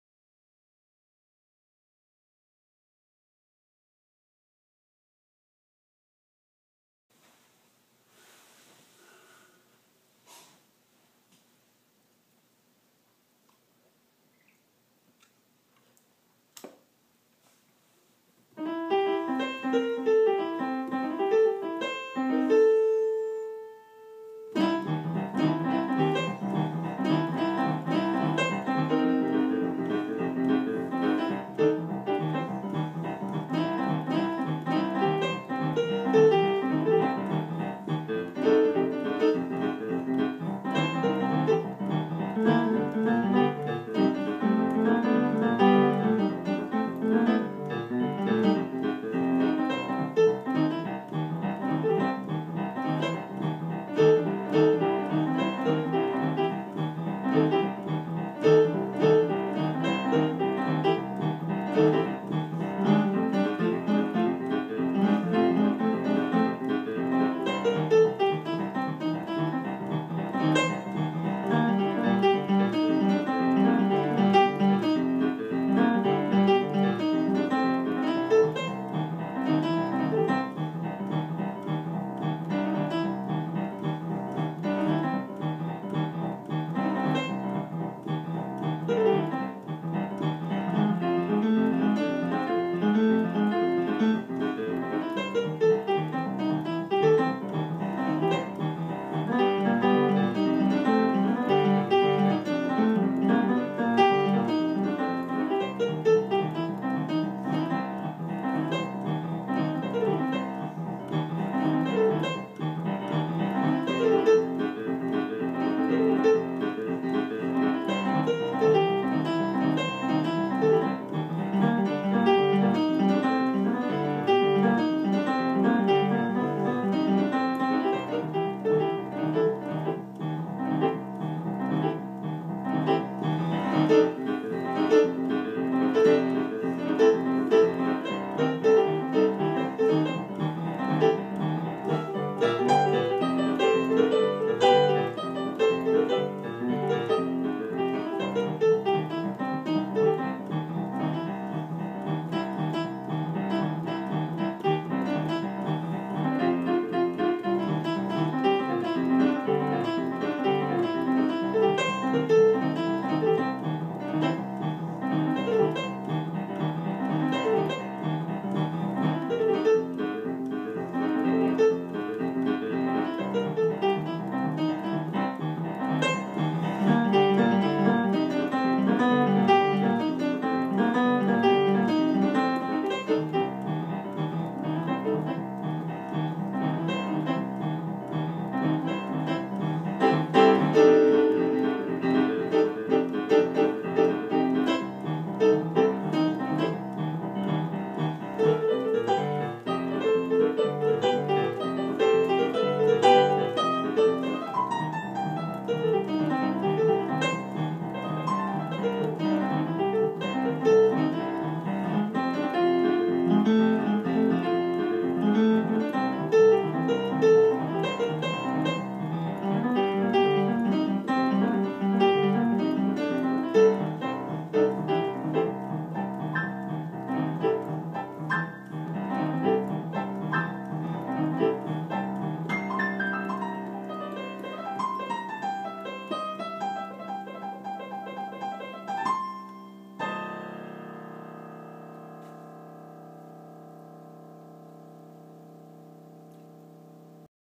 LaRue Multigun Boogie Woogie
Impromtu ivory tickling after a long nightshift.